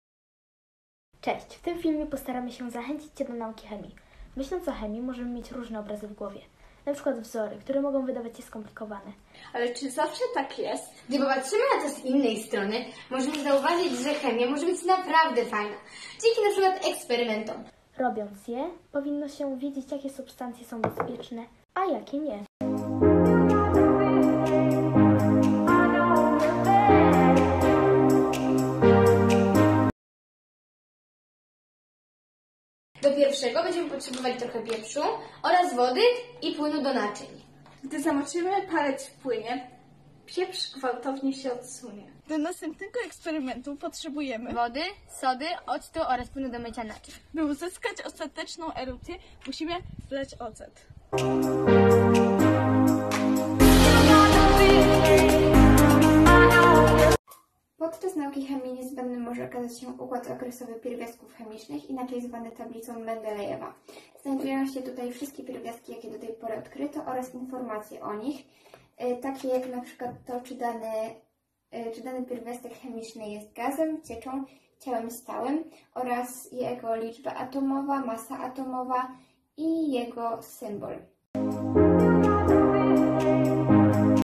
,,Laretka''
Film uczennic klasy 7, które zgłosiły się do ogólnopolskiego konkursu promującego naukę chemii ,,Z Chemią za Pan Brat'' (organizowany przez Uniwersytet Jagielloński w Krakowie). W konkursie biorą udział: Laura Reszka, Oliwia Witkowska, Antonina Kędzierska i Nela Tołwińska.